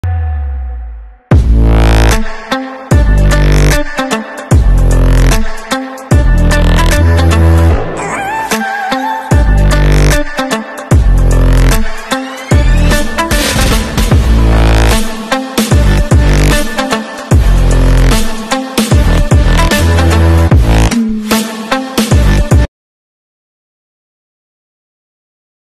DZOFILM Octopus Adapter PL-E Mount | Simple and clean combo!
Love this camera setup! Simple and clean combo🔥🎥🔥, paired with the purely mechanical Octopus Adapter PL-to Sony E-mount.